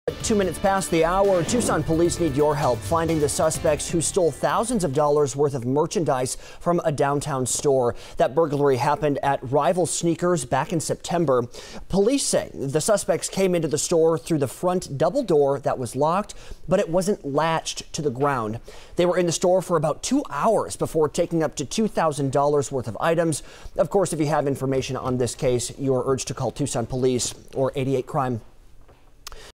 Downtown business burgarlized
An active investigation continues into finding who stole thousands of dollars worth of merchandise from Rival Sneakers.